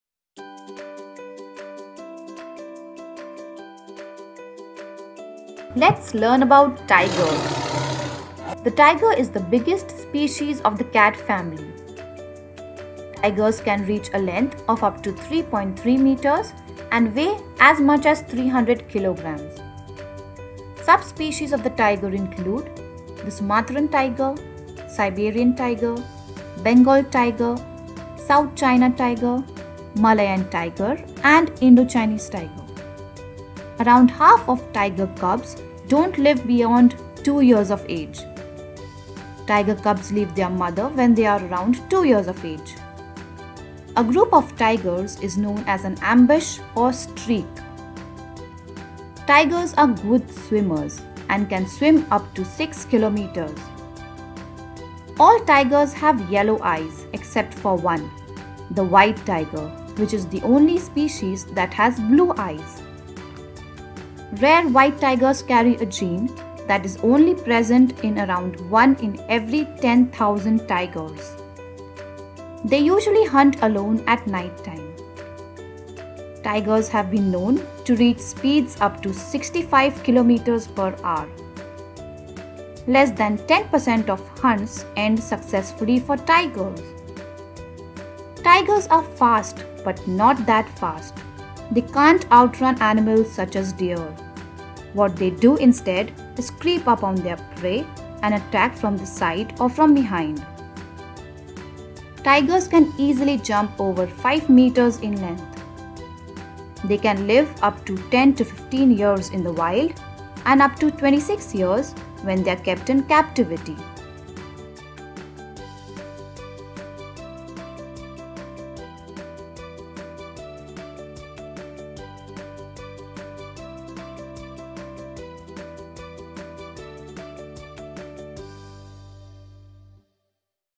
Tiger Facts for Kids | Interesting Educational Video about Tigers for Children | Fun Facts
This is an educational video for children. Here are some interesting and fun facts about Tigers.

•The tiger is the biggest species of the cat family.
•Tigers can reach a length of up to 3.3 meters and weigh as much as 300 kilograms.
•Subspecies of the tiger include the Sumatran Tiger, Siberian Tiger, Bengal Tiger, South China Tiger, Malayan Tiger, and Indochinese Tiger.
•Around half of tiger cubs don’t live beyond two years of age.
•Tiger cubs leave their mother when they are around 2 years of age.
•A group of tigers is known as an ‘ambush’ or ‘streak’.
•Tigers are good swimmers and can swim up to 6 kilometers.
•All tigers have yellow eyes, except for one; the white tiger is the only species that has blue eyes.
•Rare white tigers carry a gene that is only present in around 1 in every 10000 tigers.
•They usually hunt alone at night time.
•Tigers have been known to reach speeds up to 65 kph
•Less than 10% of hunts end successfully for tigers
•Tigers are fast, but not that fast. They can’t outrun animals such as deer. What they do instead is creep up on their prey, and attack from the side or from behind.
•Tigers can easily jump over 5 meters in length.
•They can live up to 10-15 years in the wild and up to 26 years when they’re kept in captivity.